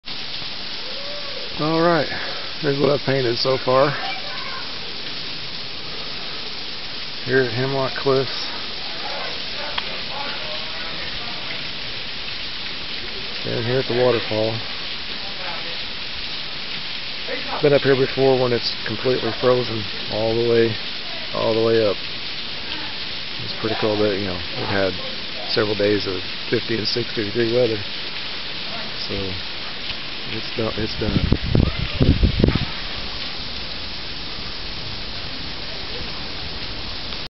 Plein Air Painting At Hemlock Cliffs
Painted today at Hemlock Cliffs in Hoosier National Forest in Southern Indiana.
Our recent snow and ice has pretty much melted away, with the temps reaching into the 60's for the past couple of days.